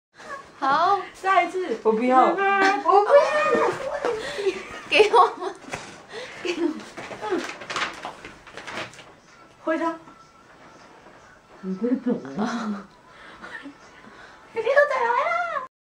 Roxane showing off a Super Junior fish!
My cousins were watching the Super Junior concert the next day so they made banners and other stuff to bring. This is one of the things they made - and according to them, Super Junior's member (whose face appears on the fish) saw it and gave them the thumbs up!

They were sooo happy! :)